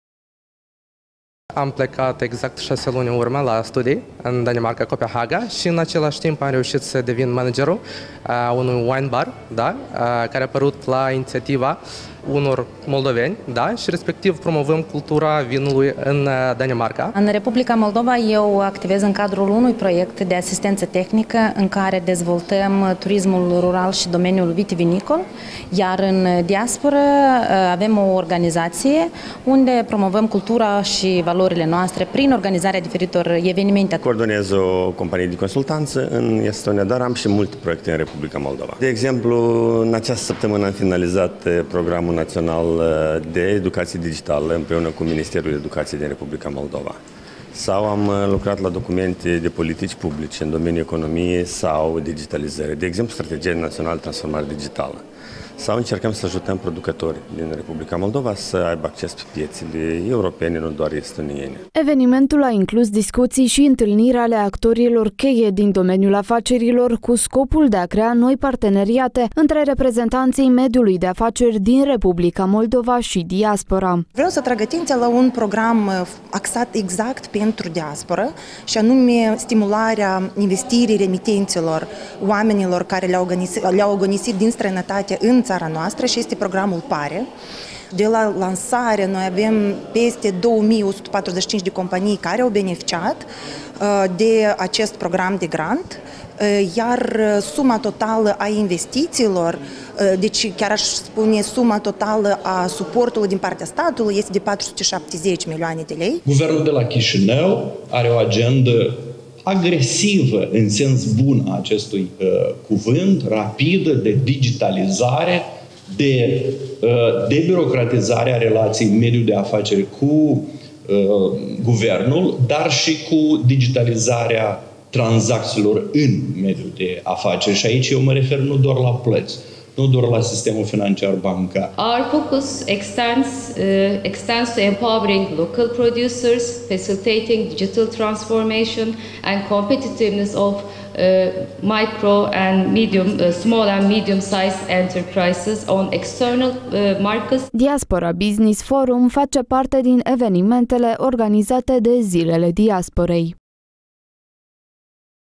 Oportunități de dezvoltare a afacerilor, la Diaspora Business Forum
Antreprenori şi profesionişti basarabeni din domeniul economic plecaţi peste hotare şi-au dat întâlnire astăzi la Diaspora Business Forum 2024, care a avut loc la Chişinău. S-a discutat despre oportunităţile de dezvoltare a afacerilor în Republica Moldova şi s-au prezentat poveşti de succes în domeniul antreprenorial.